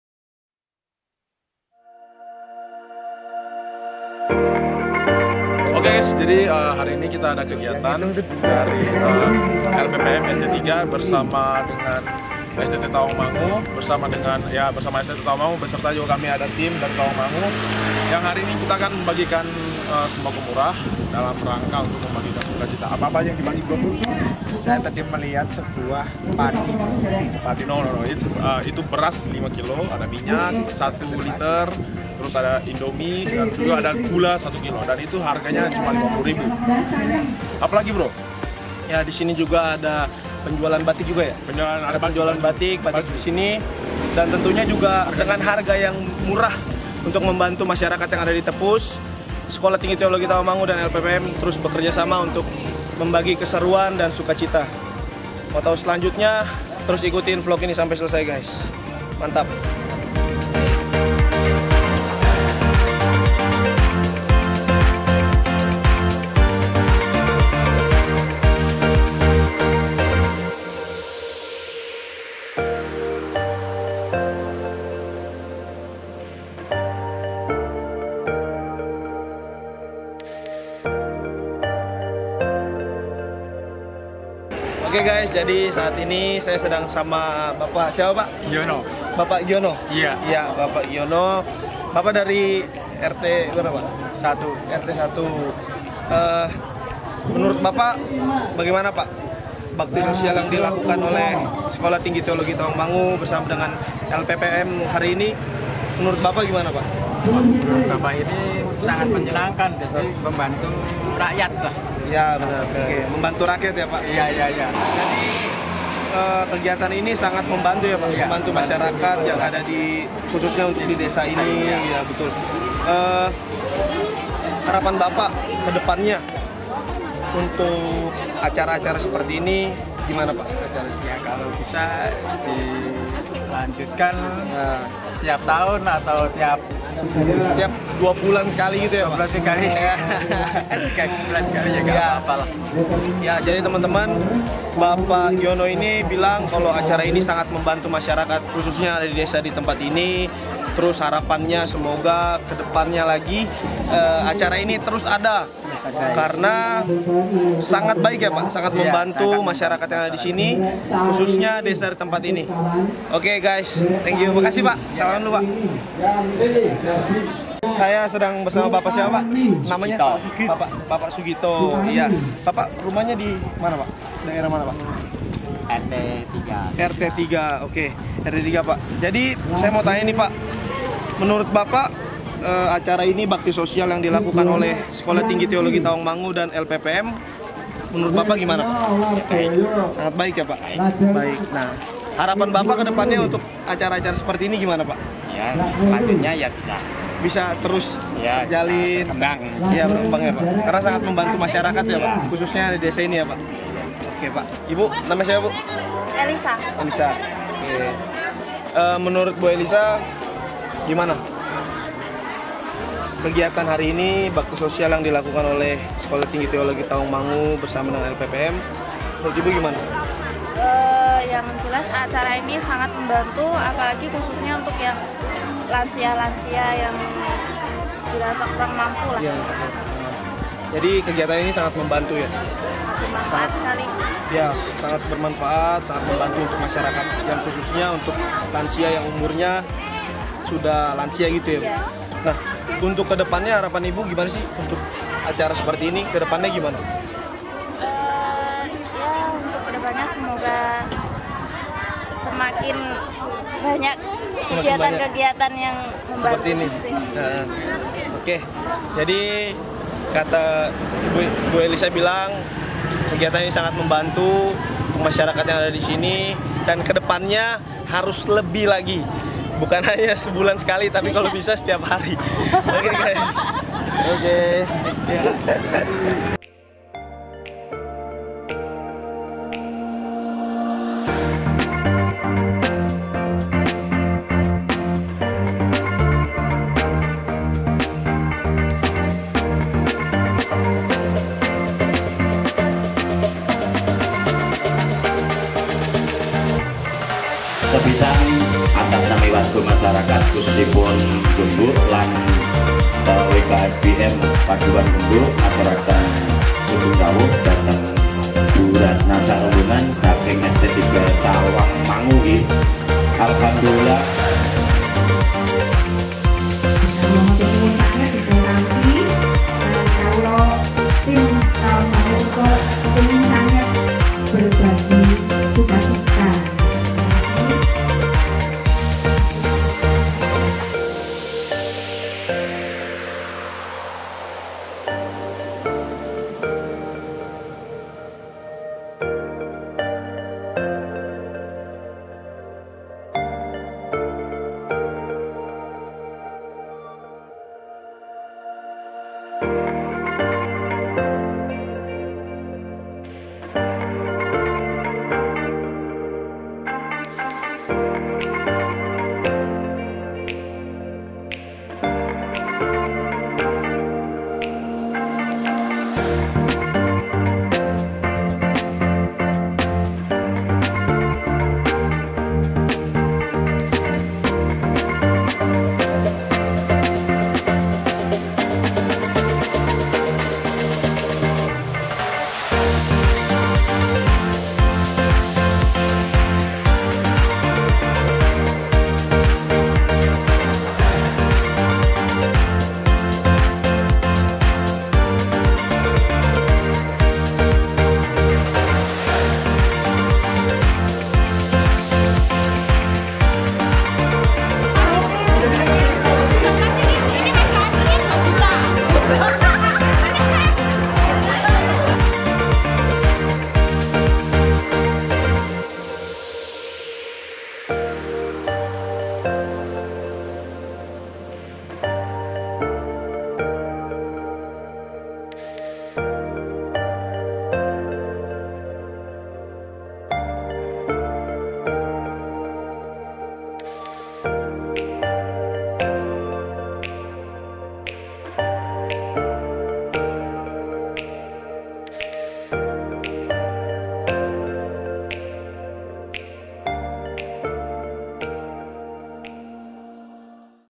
Mengadakan Pasar Murah bersama LPPM (Lembaga Penelitian Pengabdian Masyarakat)
Lembaga Penelitian dan Pengabdian Kepada Masyarakat (LPPM) adalah payung institusi bagi Pusat Penelitian (Puslit) dan Pusat Pengabdian kepada Masyarakat (PPM).
Nah, di STT Tawangmangu sendiri kegiatan demi kegiatan pemasyarakatan ini juga akan terus diadakan.